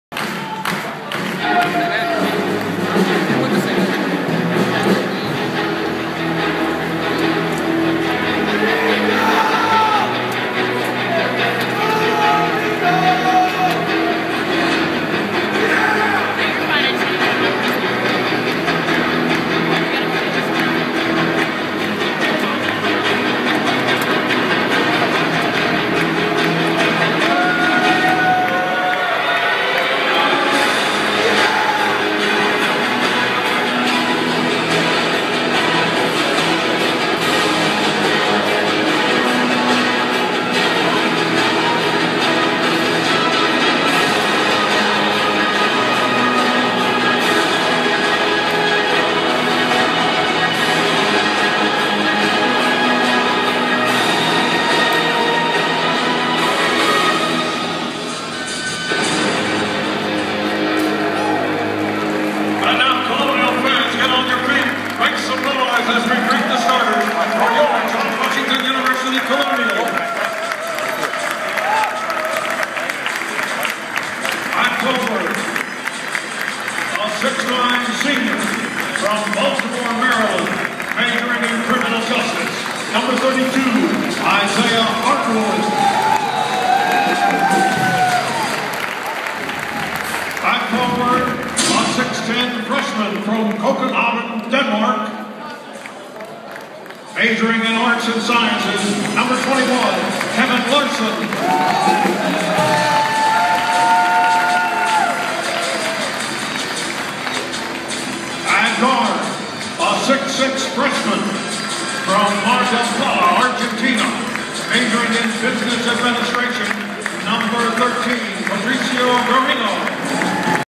Presentacion de George Washington University
La presentacion de GWU con Pato Garino en sus filas.